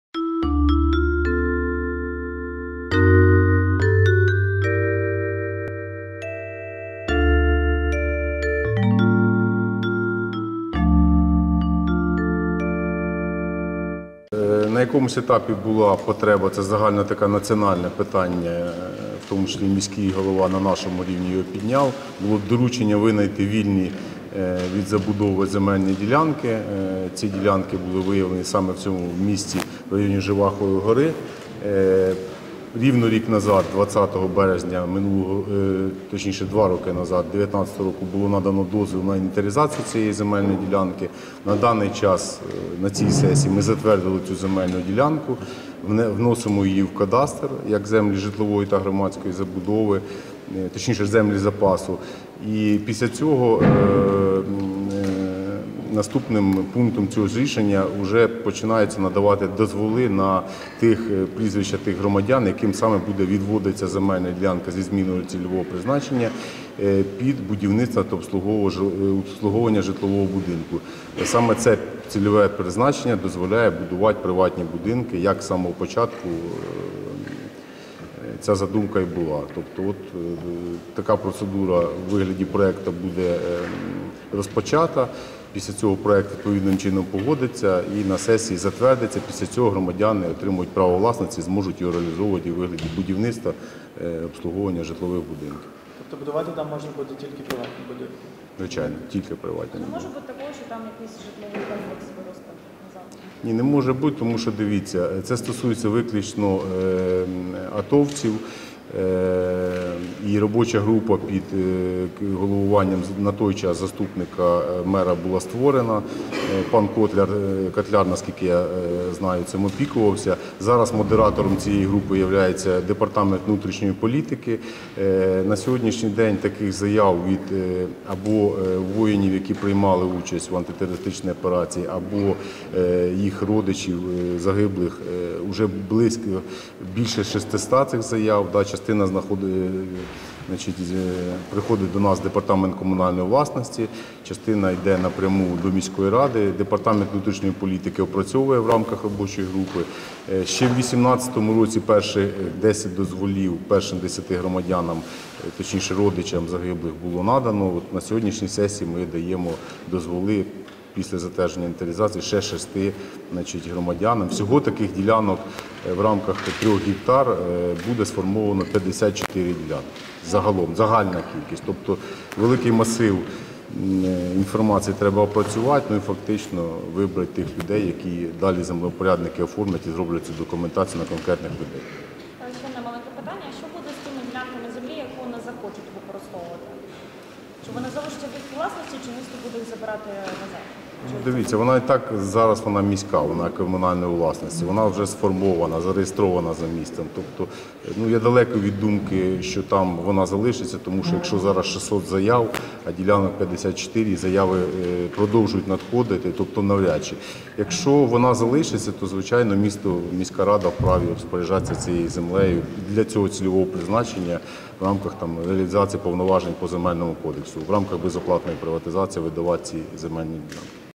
Щодо відведення земельних ділянок на території Жевахової гори
Коментар в.о. заступника директора Департамента комунальної власності Одеської міськради Олександра Філатова.